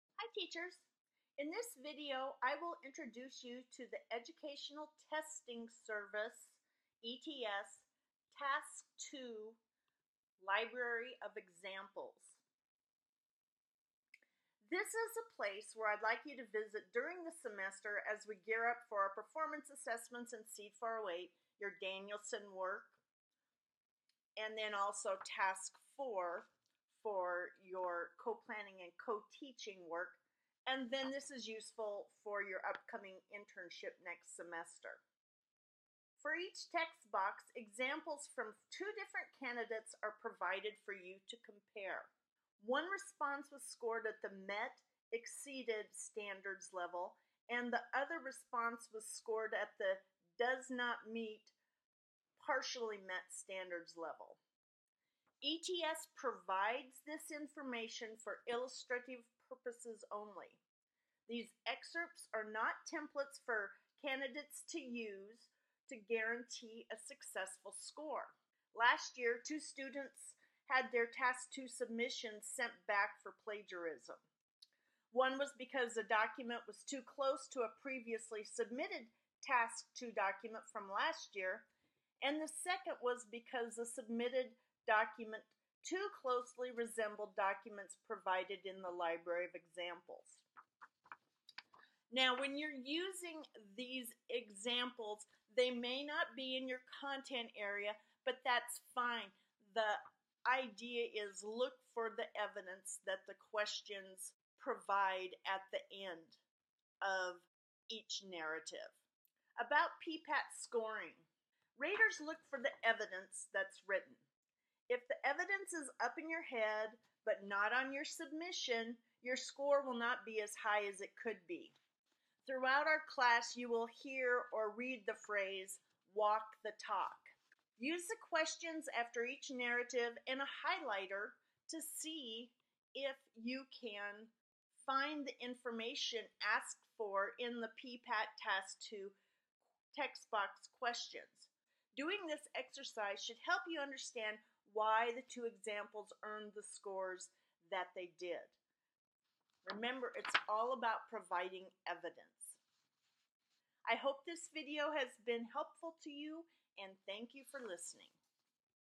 PPAT Task 2 Library of Examples
In this video, I give an overview of what you can find in the ETS PPAT Library of Examples. The video focuses on task 2 Assessment and Data Collection to Measure and Inform Student Learning